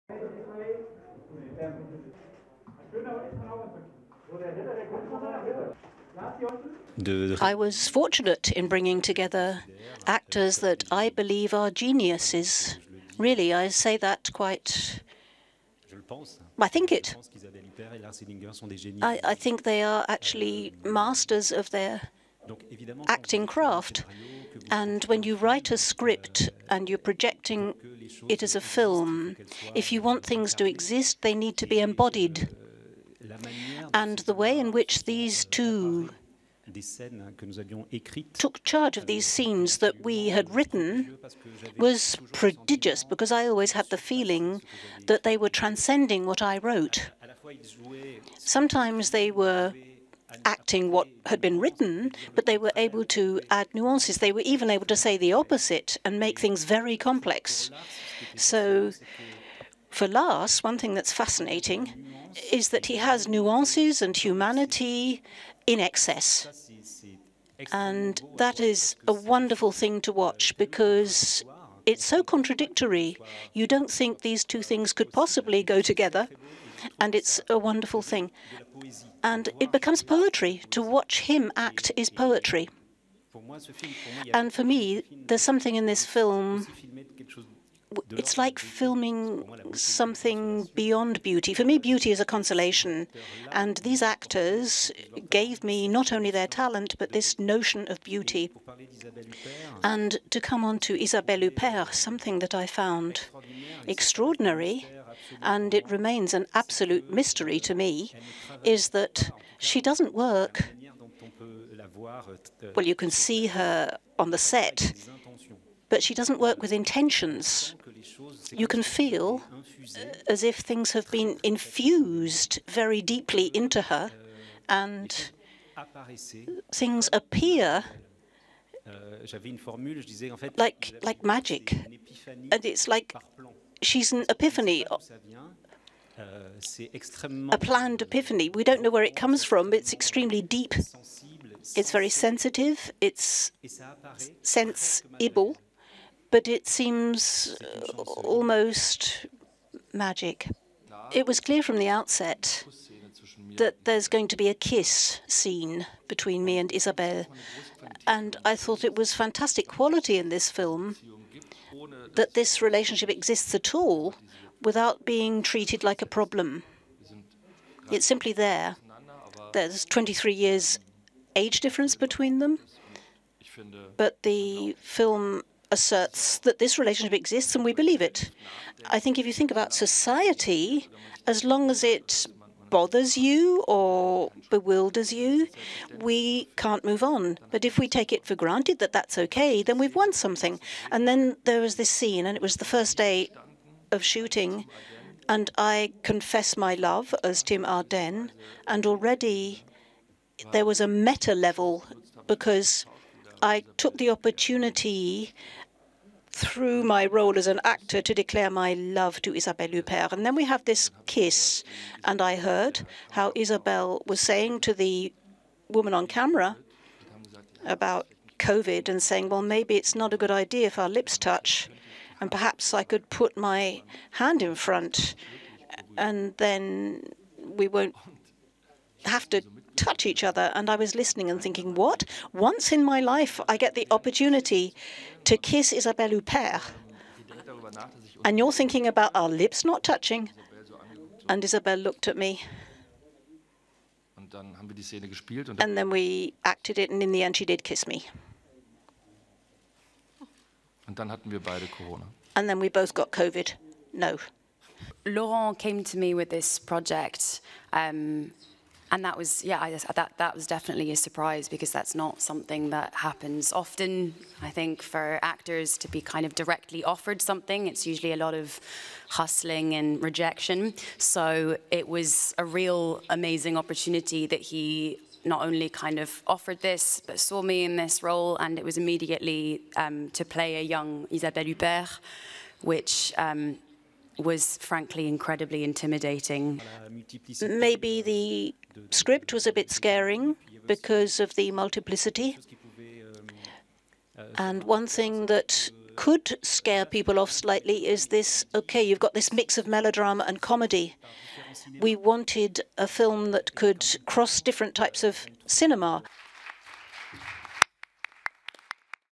About Joan | Press Conference Highlights | Berlinale 2022
Press conference highlights for "About Joan" (À propos de Joan) with: Laurent Larivière (director), Freya Mavor and Lars Eidinger (actors). Tuesday, February 15, 2022 at the Berlinale Press Centre.

Part of the Berlinale's 2022 Competition.

About the film: Successful publisher Joan Verra stares into the camera and begins to tell her story: one liberated woman’s nostalgic, fragmented and dreamlike autobiography, set between Ireland and France, between partings and returns, between pain inflicted and received.